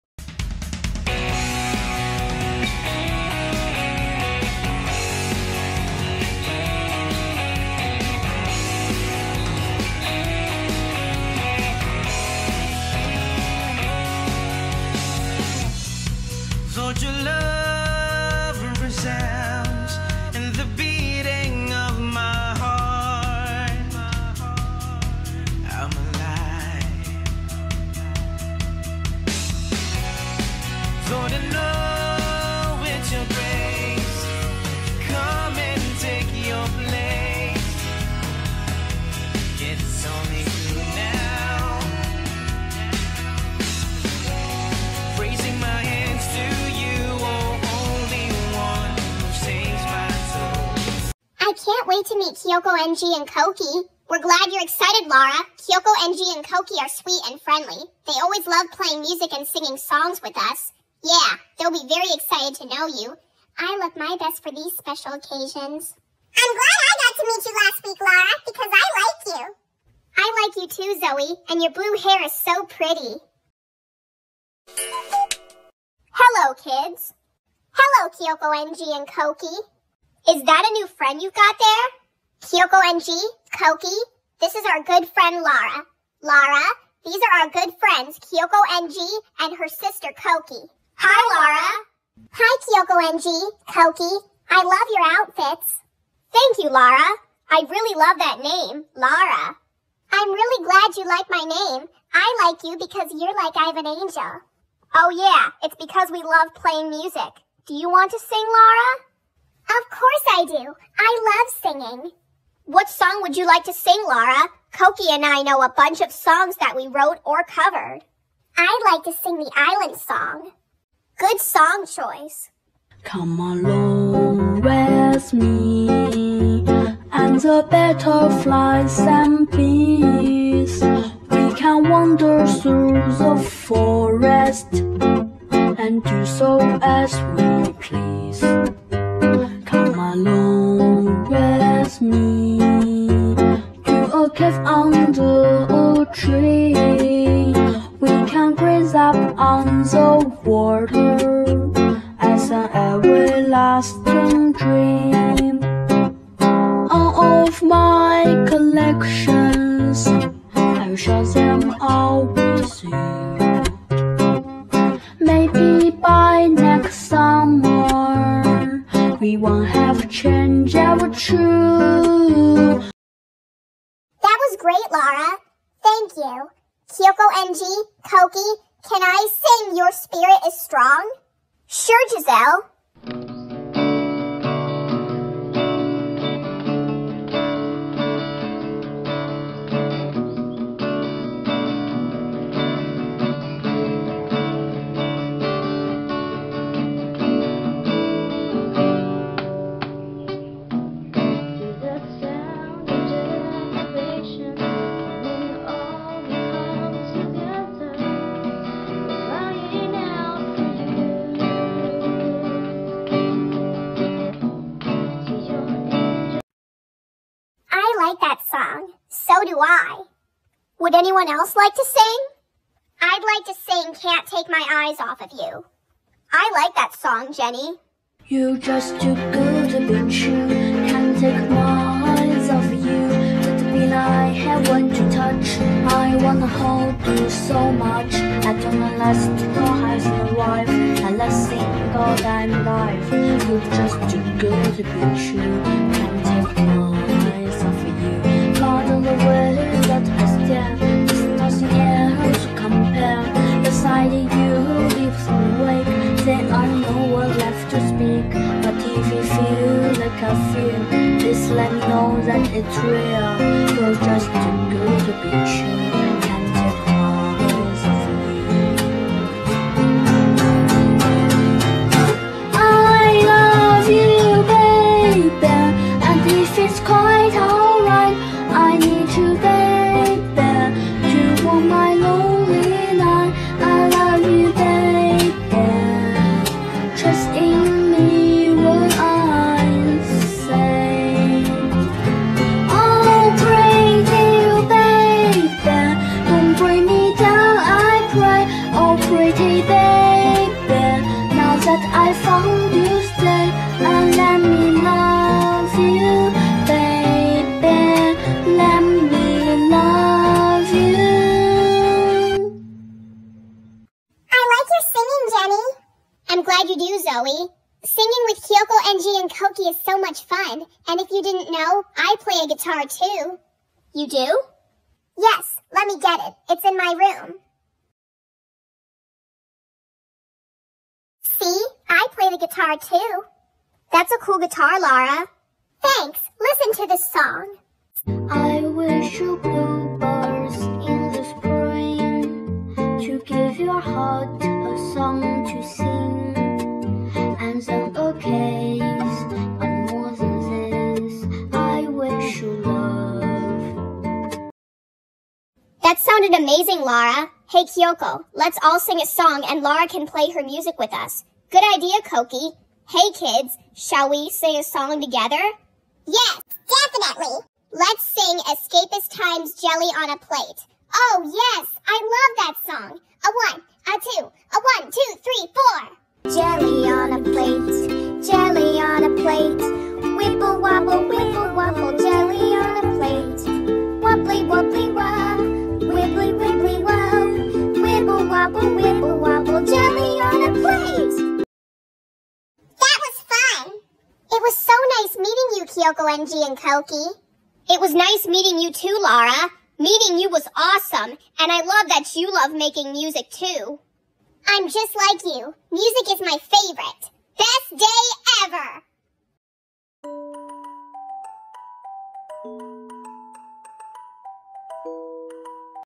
TSPA || episode 13 || Laura meets Kyoko Ng and Kokie Gacha Club
Me, Jaeselle, Jenny, Zoe, and Max introduce Laura to Kyoko Ng and Kokie. We sing songs and have fun